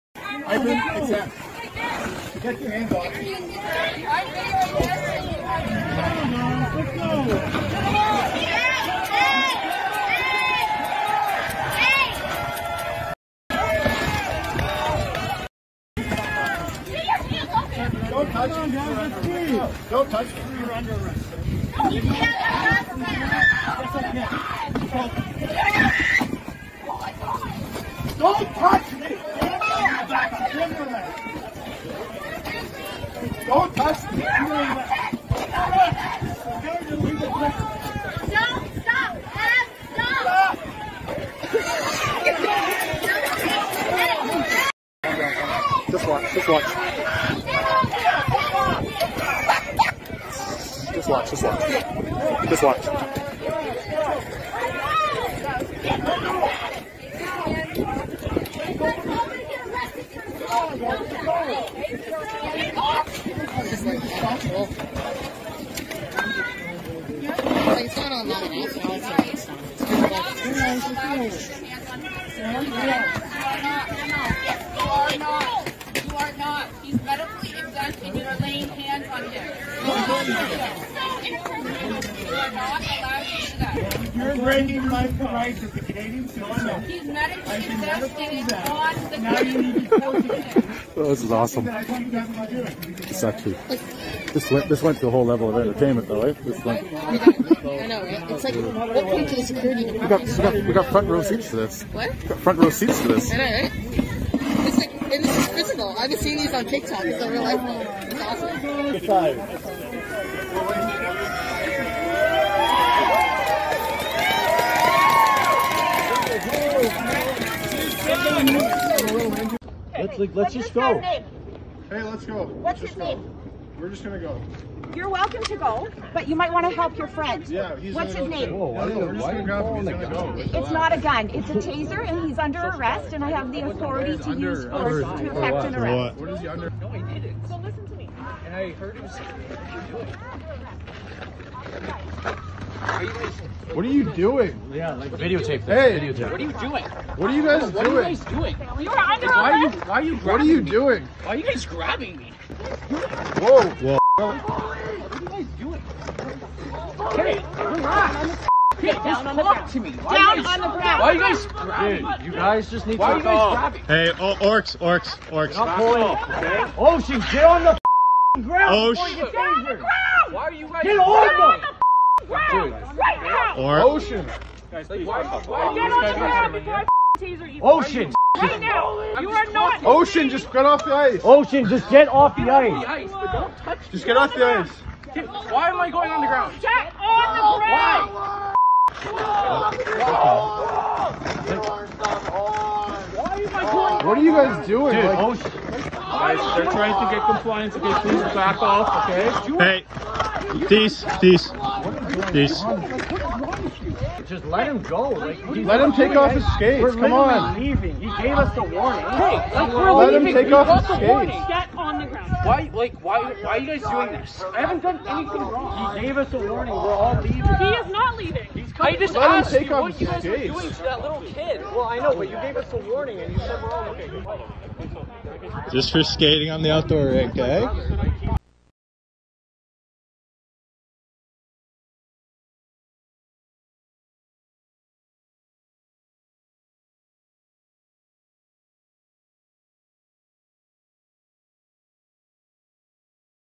Canadian Man Battles Security Over Refusal To Wear Mask On A Ski Lift
A man who wouldn't wear a mask before getting on a ski lift stumbles into a battle with security.